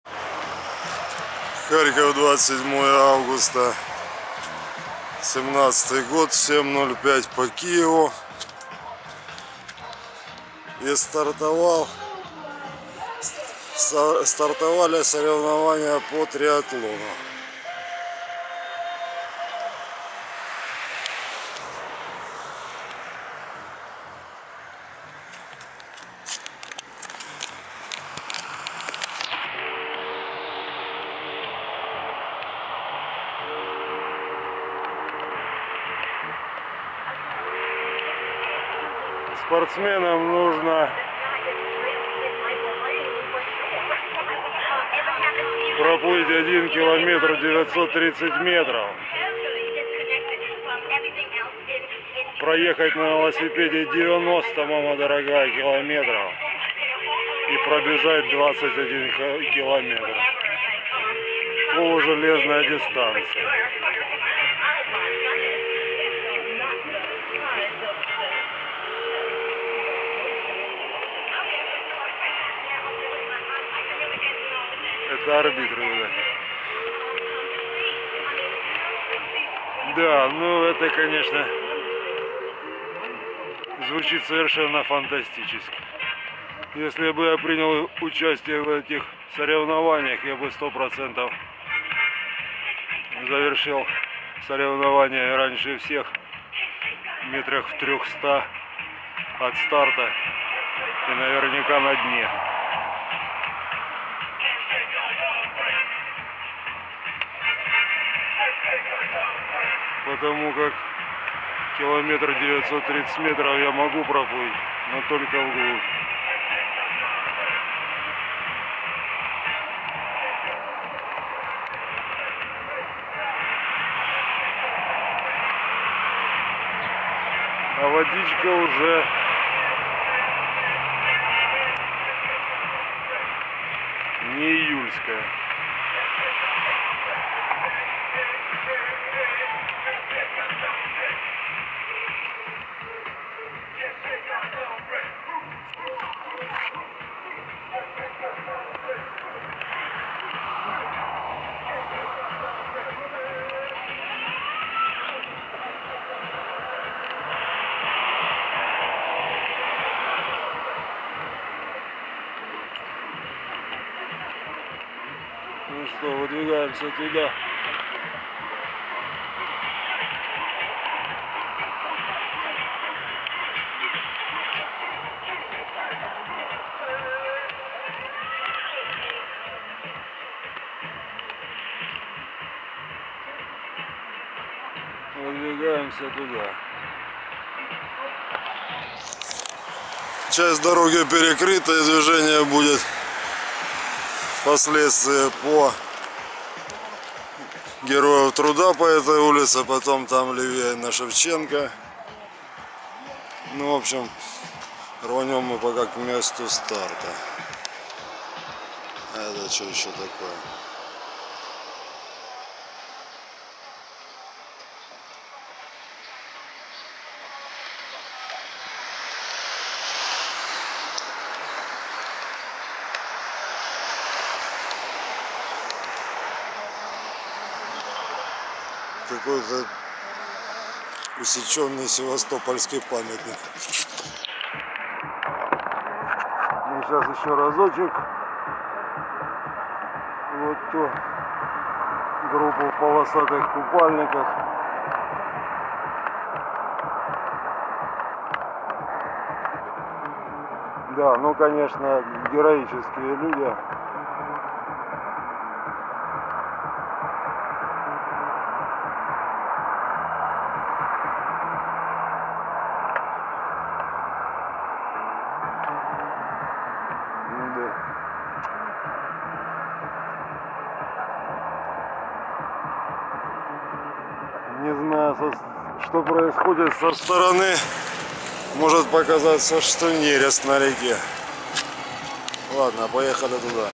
Триатлон «Iron Way» в Харькове 27 августа 2017 заплыв 1930 метров
27 августа 2017 года в Харькове проходят соревнования по триатлону: дистанция заплыва 1930 метров (плюс 90 километров велозаезд, плюс забег на 21 километр.